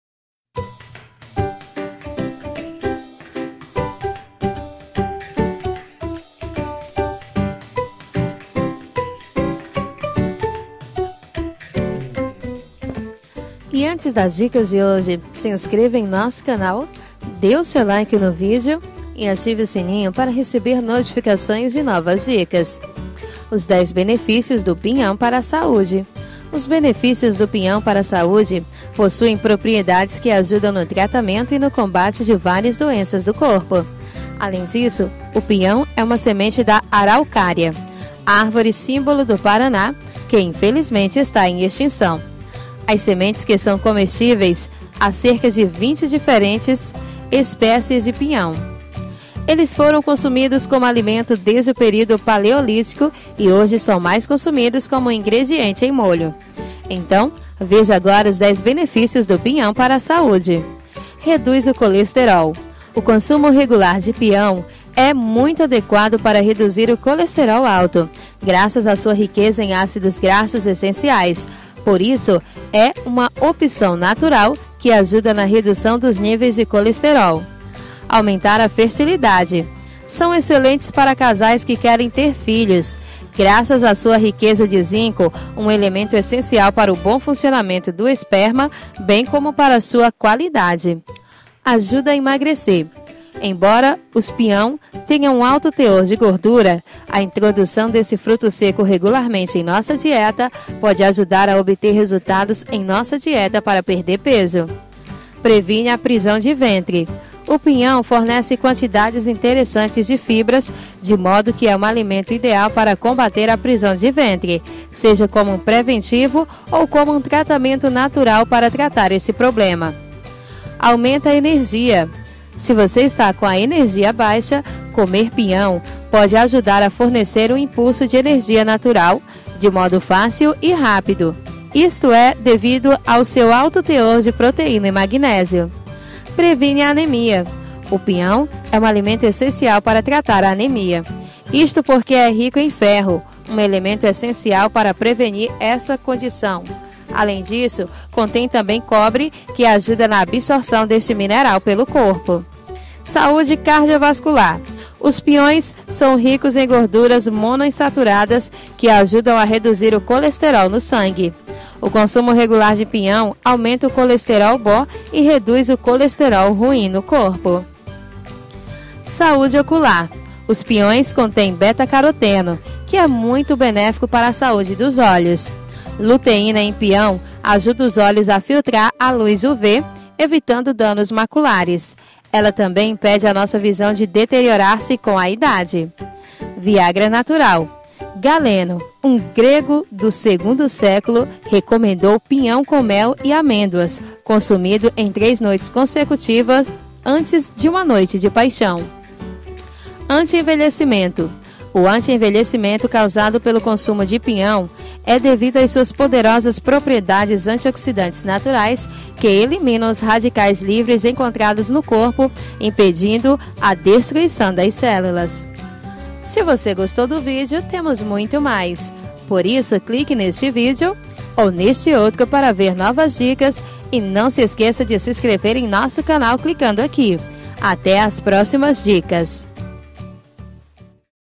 Os 11 Benefícios do Pinhão Para Saúde | Dicas de Saúde
O Pinhão reduz o colesterol e aumenta a fertilidade  para casais que querem ter filhos .Então, confira Os 11 Benefícios do Pinhão Para Saúde!

Os Benefícios do Pinhão Para Saúde possui propriedades que ajudam no tratamento e no combate de várias doenças do corpo.

Se você não sabe, o Pinhão também é uma excelente fonte de vitaminas, tais como:

- vitamina C,
- Vitamina B,
- vitamina A
- proteínas e minerais como:
- cálcio,
- fósforo,
- ferro,
- potássio,
- zinco.

As pessoas também perguntam isso sobre o Pinhão:

- Quais os benefícios do pinhão para a saúde?

- Qual o valor do Pinhão?

- Onde tem pinhão?

- Quem é diabético pode comer pinhão?